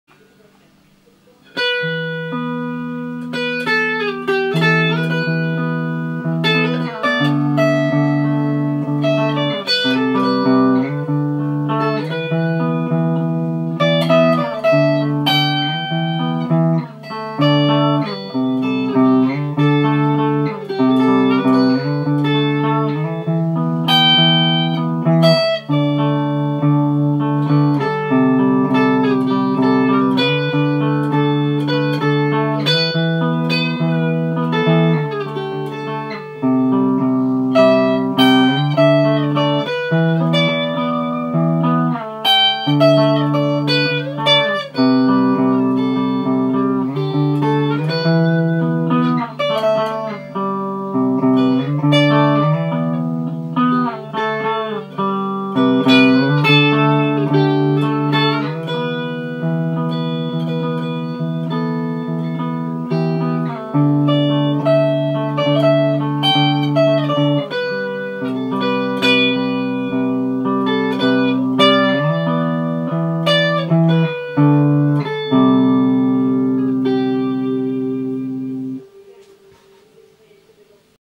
Loop pedal jam - Mat. Oldershaw
Messing around with a loop pedal for the first time.